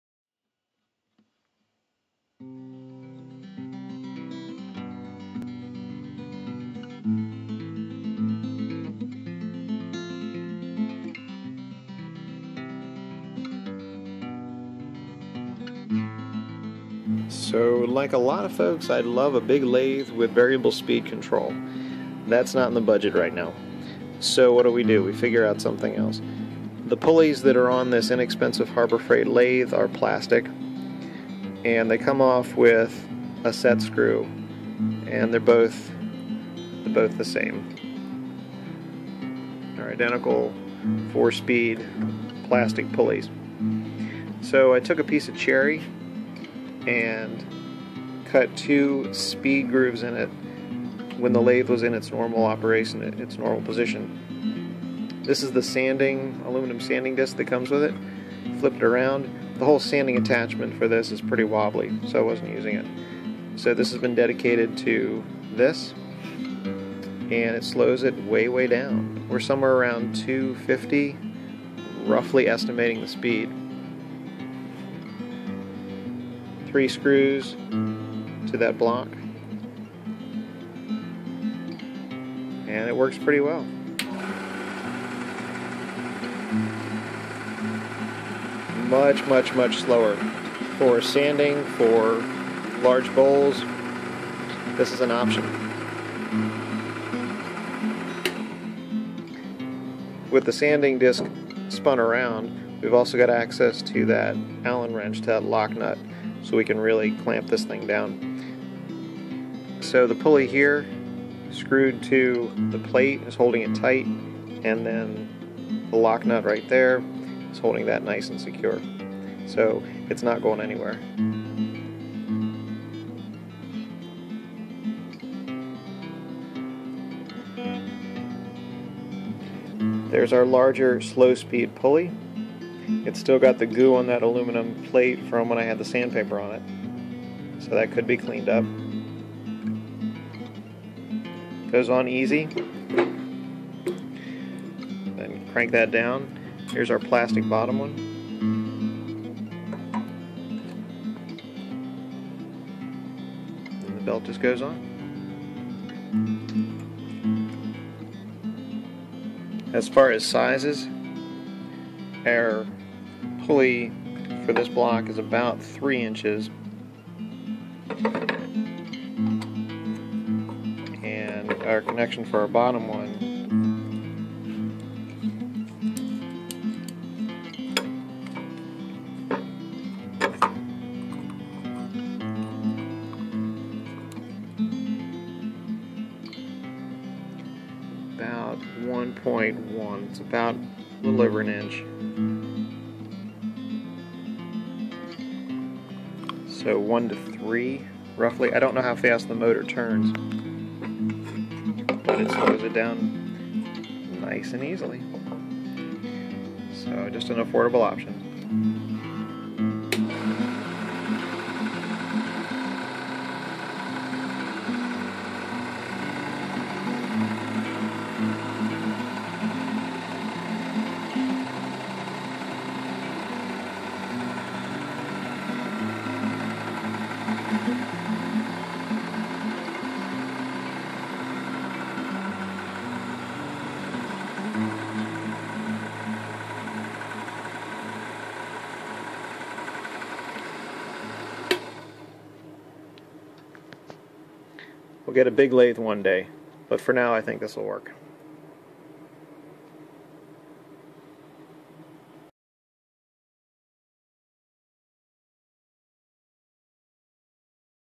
How to Slow Down A Basic Lathe (A DIY Mod)
I have a very basic Harbor Freight lathe.  It works pretty well for the cost and can produce some cool turnings.  But for larger bowls that are heavy or very out of balance when first put on the lathe a slower speed is needed.  The slowest this lathe was designed to go with the factory pulleys is around 900 rpm.  With this little mod I've dropped the speed by about 250+/- (based on some rough estimation).  After a bowl is cleaned up and balanced this mod can be removed and the regular (plastic) pulleys put back on.  Thanks for taking a look.